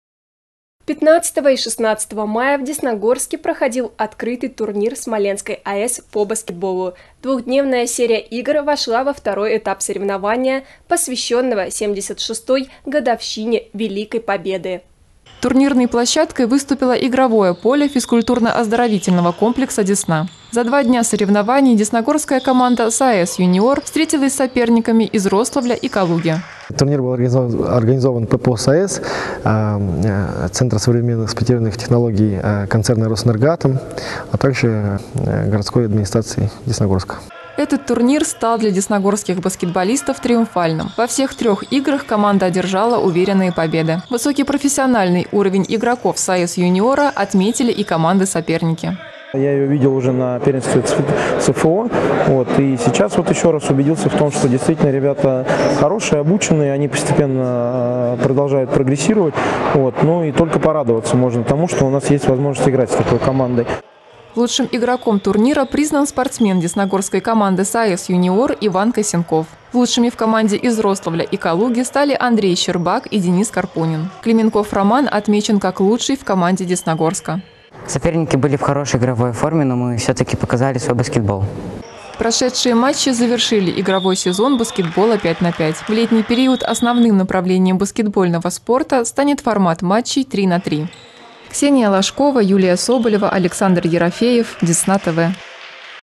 Десна-ТВ: В Десногорске прошёл двухдневный баскетбольный марафон
15 и 16 мая в Десногорске проходил Открытый турнир Смоленской АЭС по баскетболу. Двухдневная серия игр вошла во второй этап соревнования, посвященного 76-й годовщине Великой Победы.